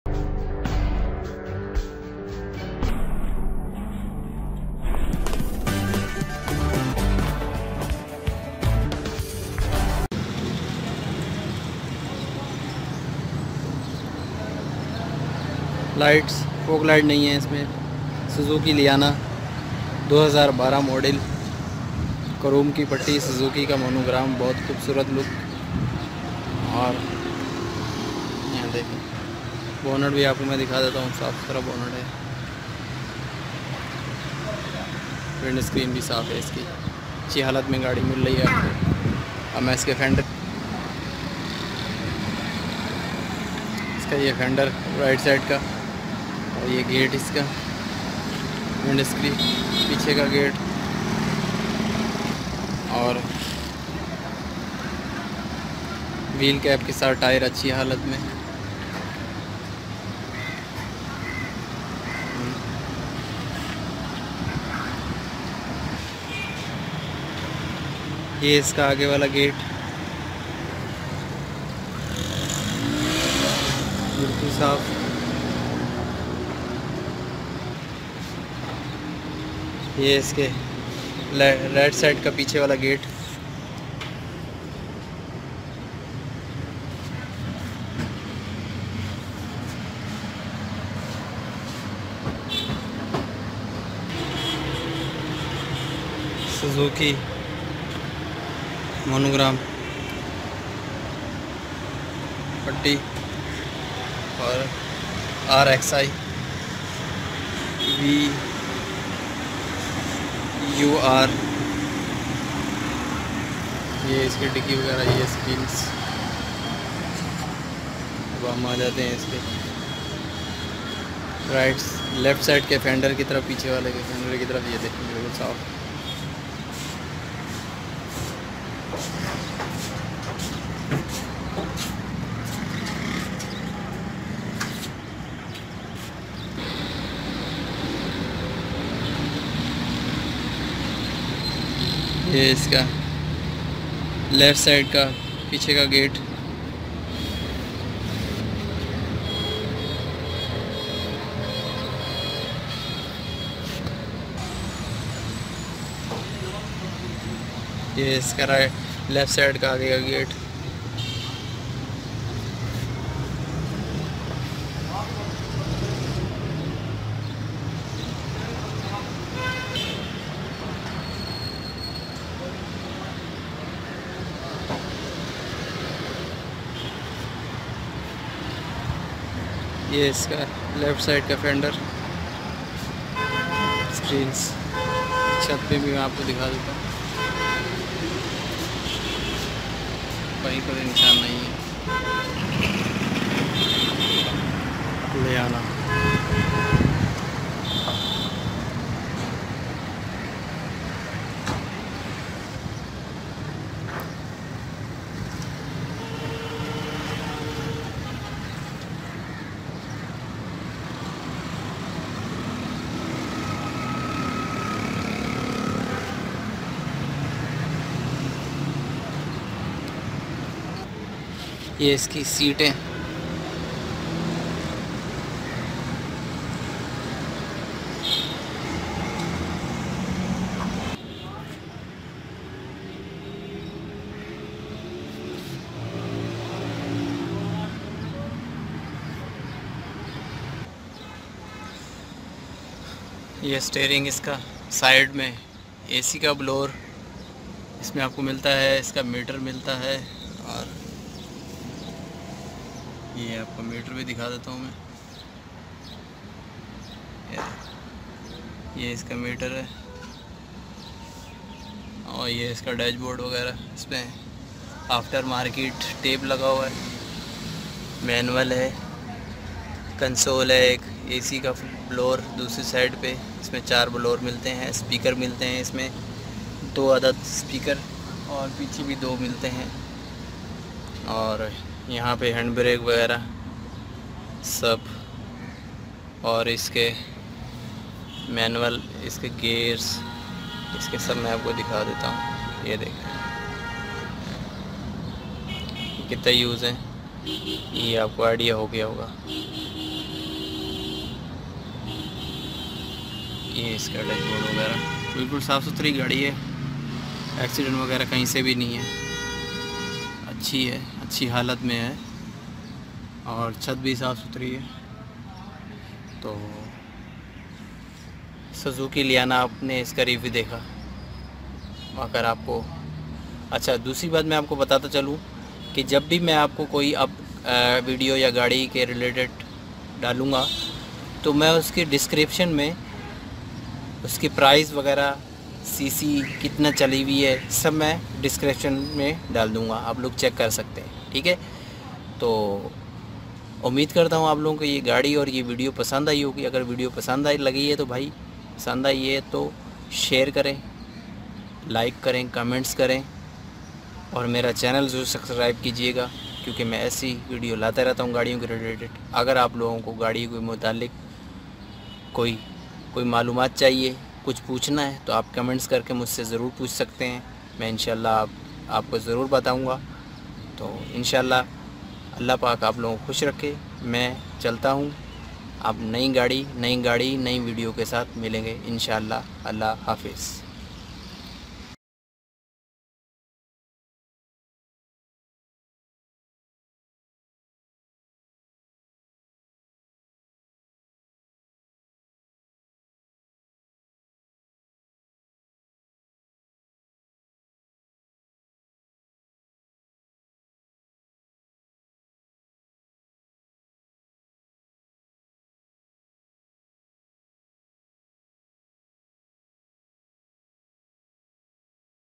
Suzuki Liana 2012 Complete Review
suzuki liana
48.000 km
petrol
Rs.875.000